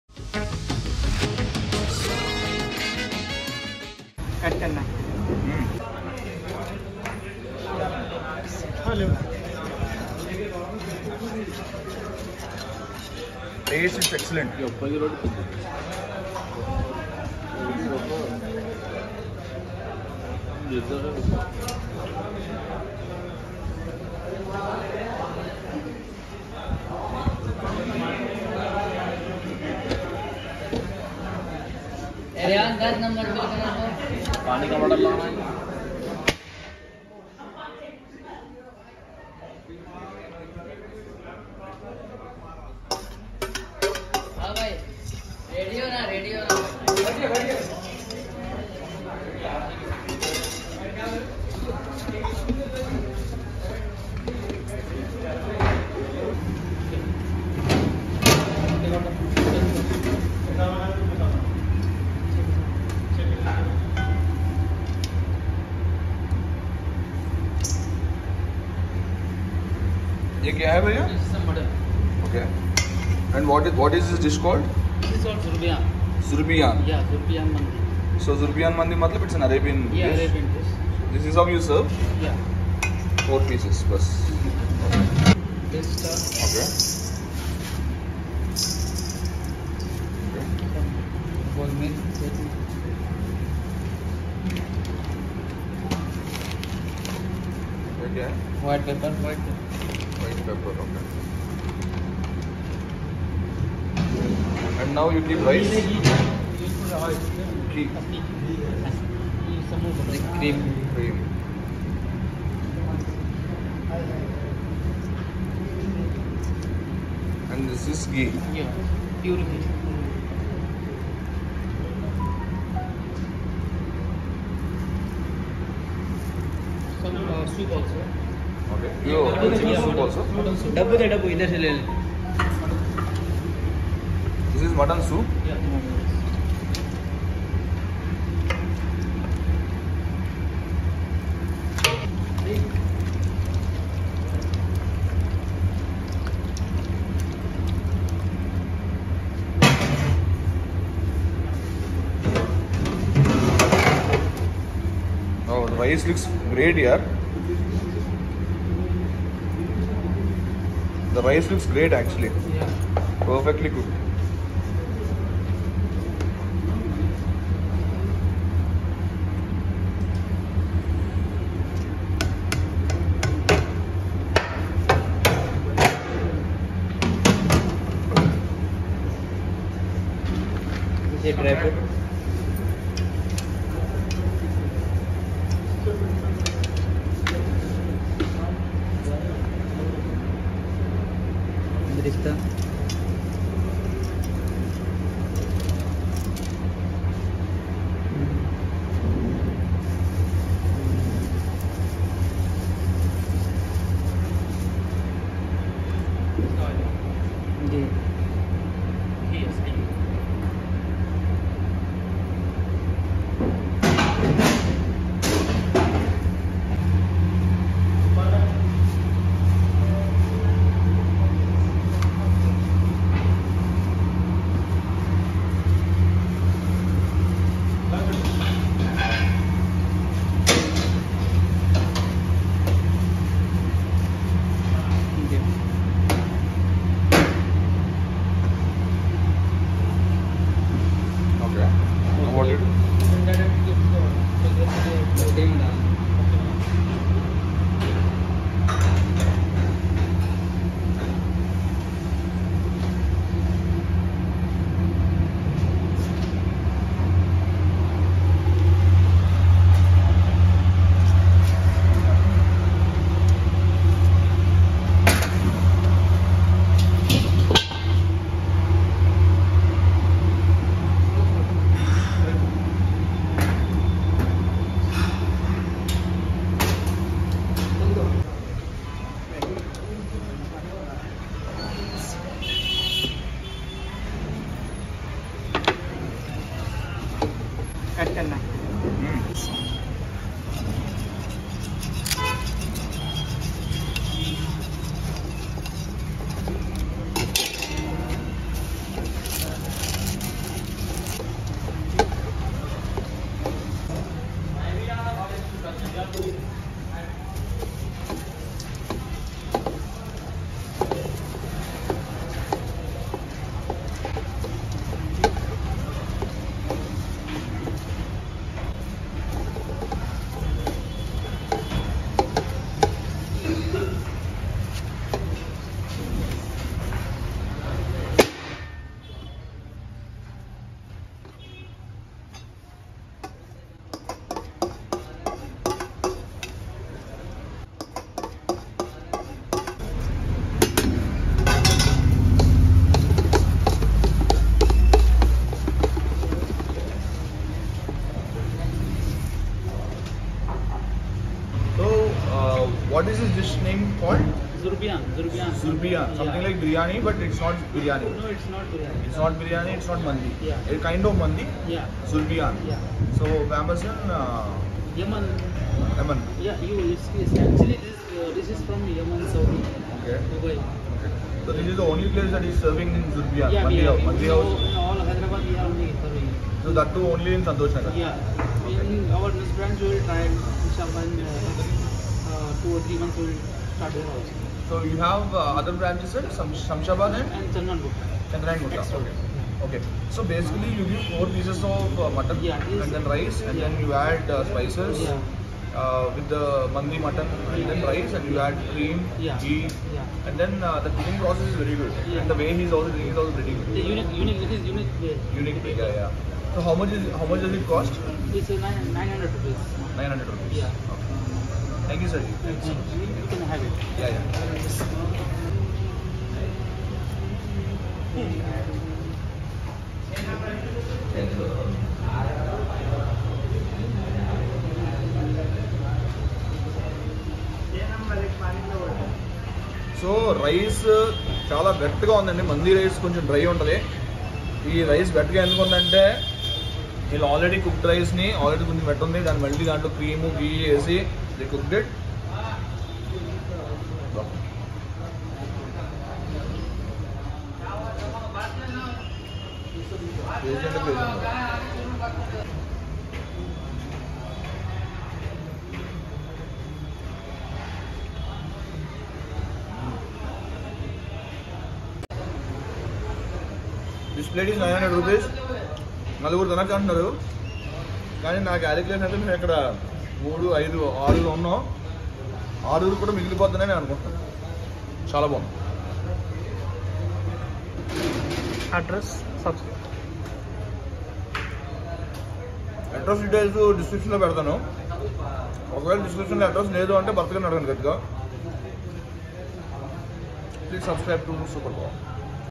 Zubriyan Mandi Making | new Concept Mandi in Hyderabad | Hyderabad Food | Street Byte | Silly Monks
Restaurant name : Mandi House Arabian restaurant
Our branch : chandrayangutta x road..
Shamshabad Airport road ..
Saidabad maim road ..